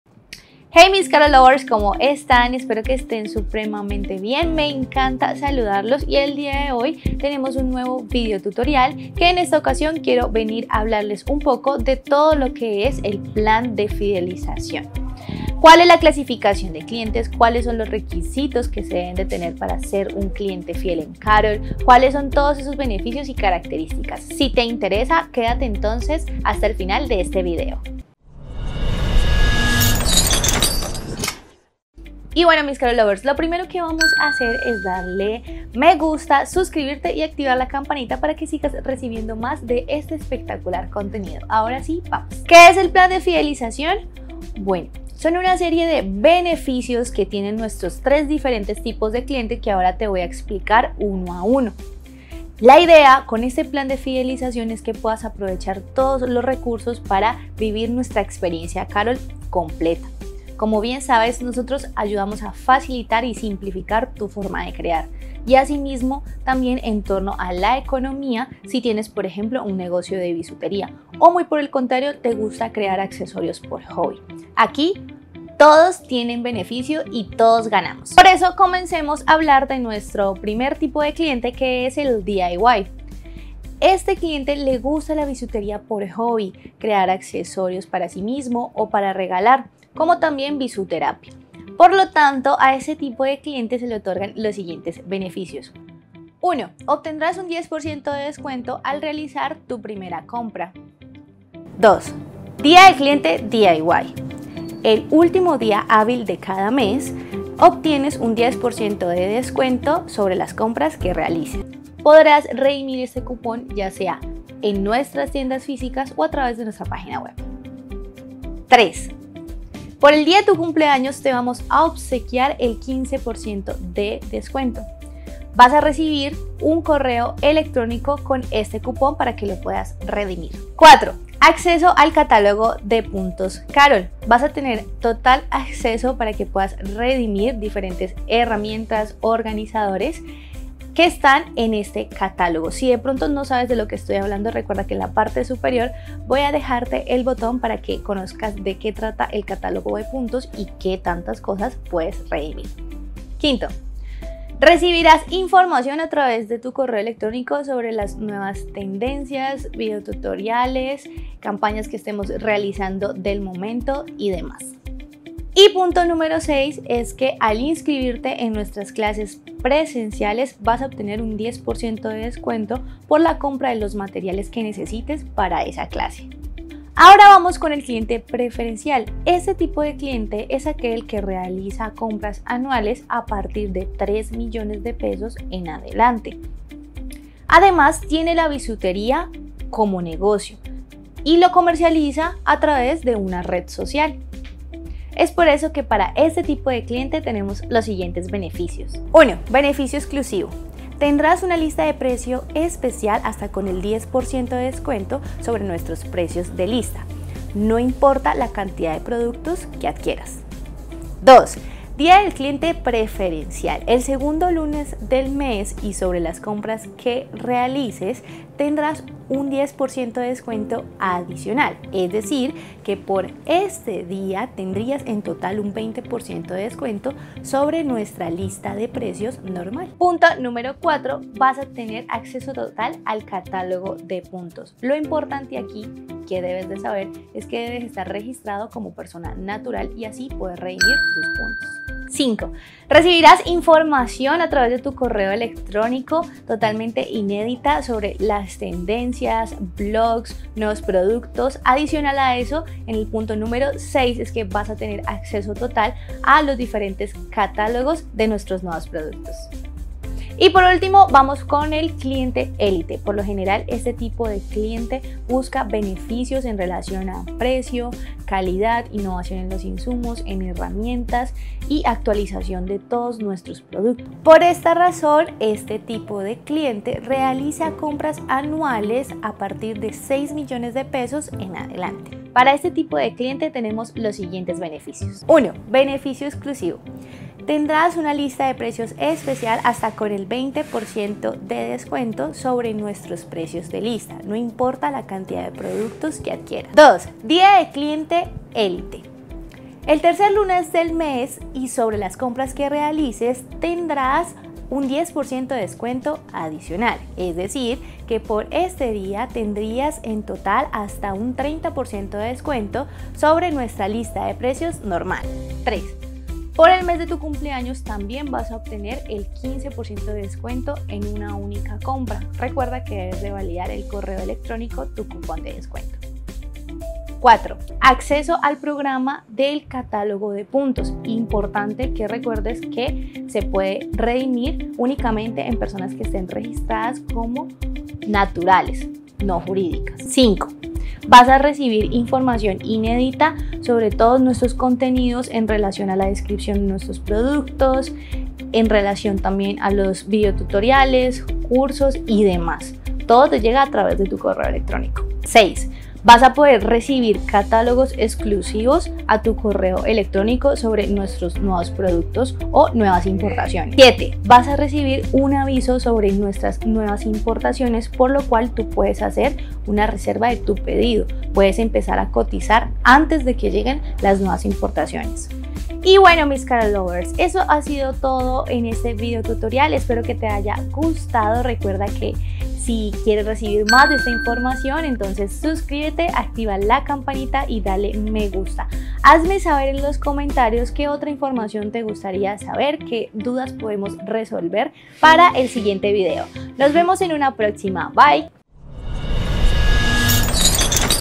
¿Cómo ser un CLIENTE FIEL EN CAROL? ⭐️⭐️⭐️
Quieres entender los diferentes precios 💲 de la página web 💻?

⭐️ En este vídeo te aclaramos muchas dudas sobre las tres opciones de precio💲 que te aparecen en la página web 💻 al momento de realizar tus compras🛒.
⭐️A que precio💲 tienes acceso de acuerdo a la categoría de clientes💻
⭐️Con diferentes ejemplos te mostramos a que precio 💲comprar tus insumos favoritos
⭐️Mira este vídeo hasta el final y déjanos todas tus inquietudes y sugerencias en los comentarios